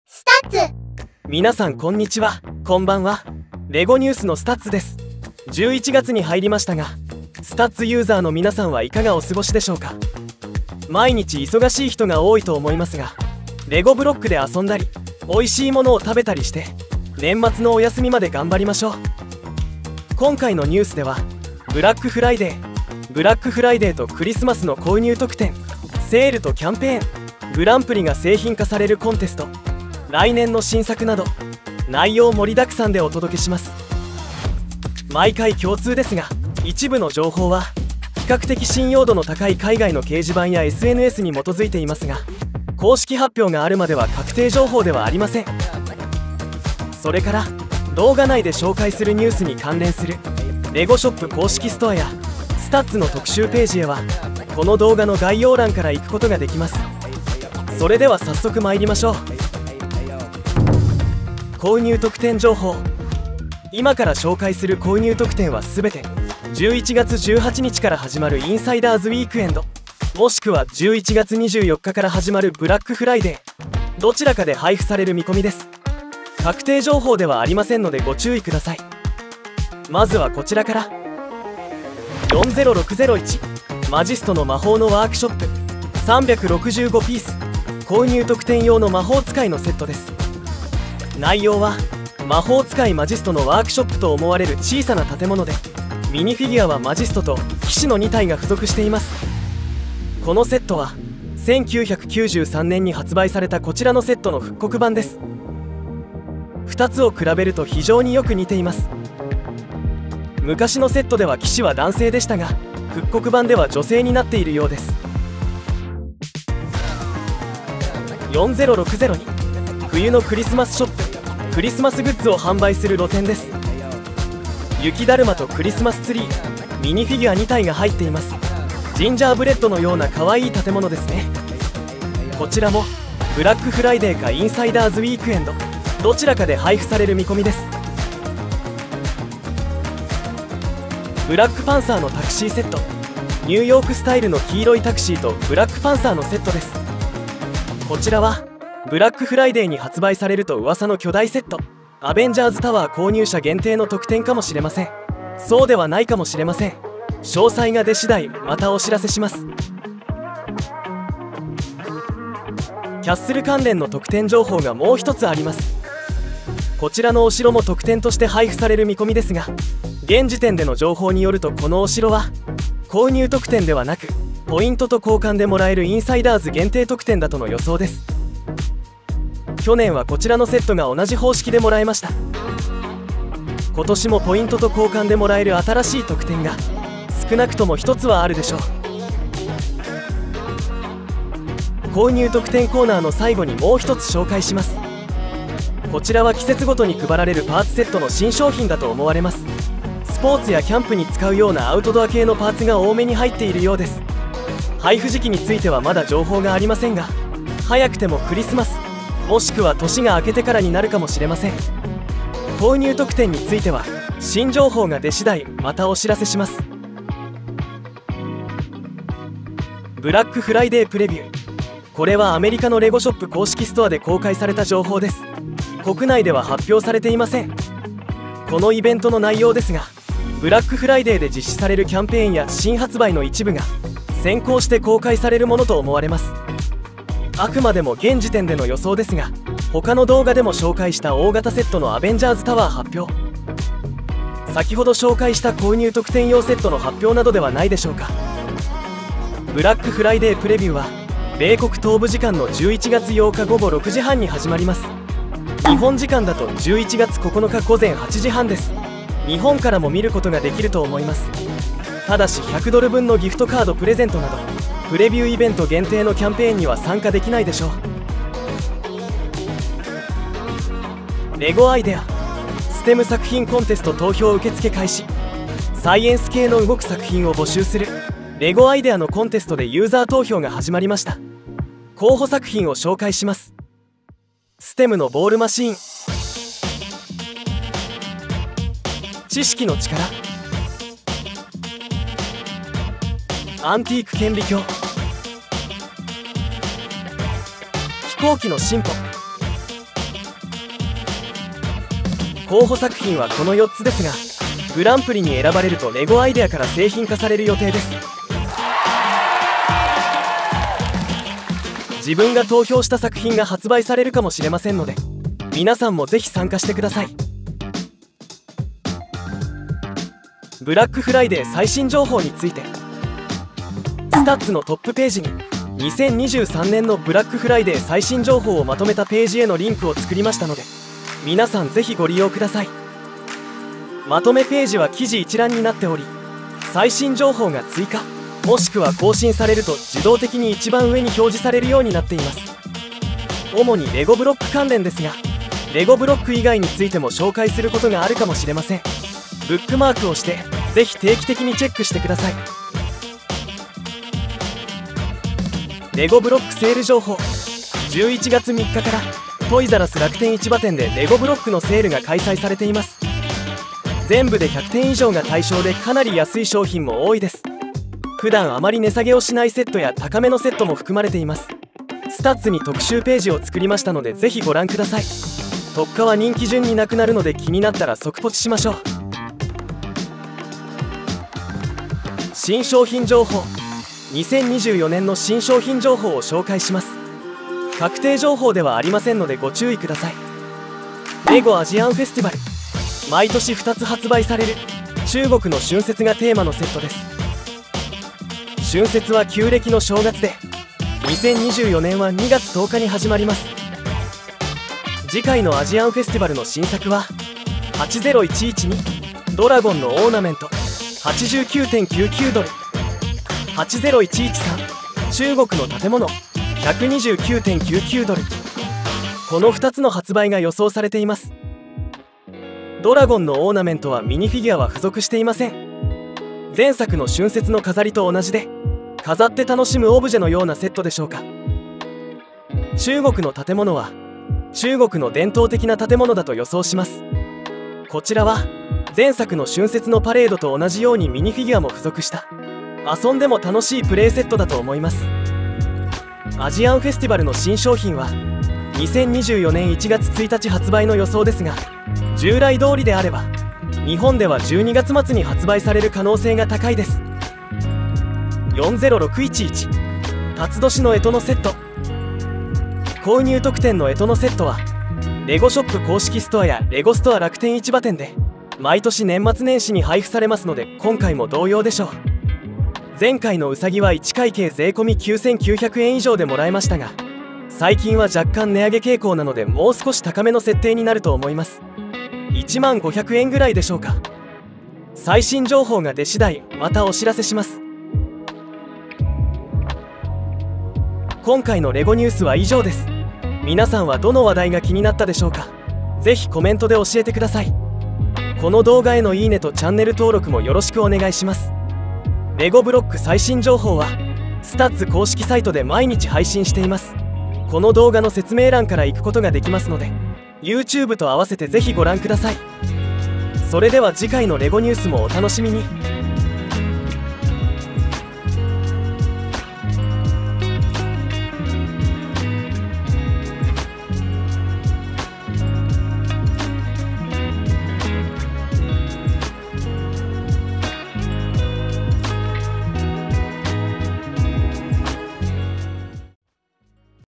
レゴ(R)ニュース/2023ブラックフライデーとクリスマス購入特典 / 2024春節新作 / ビッグセール、グランプリ製品化STEMコンテスト情報などまとめて一挙に紹介
2023年ブラックフライデーとクリスマスの購入特典、2024年新作、レゴ(R)ブロックセール、グランプリが製品化予定のSTEMコンテスト情報などまとめて紹介。

ブラックフライデー最新情報(随時更新)

🔽動画目次
0:00 今回の動画内容説明
1:00 購入特典情報
3:39 ブラックフライデープレビュー
4:31 STEMコンテスト
5:20 ブラックフライデーブラックフライデーのリンク
5:55 トイザらス楽天市場店セール
6:22 2024年新商品情報
8:11 最新情報はスタッズで更新

1番人気！レゴ(R)ブロックキャンペーン最新情報

レゴ(R)ブロック新商品・新作情報まとめ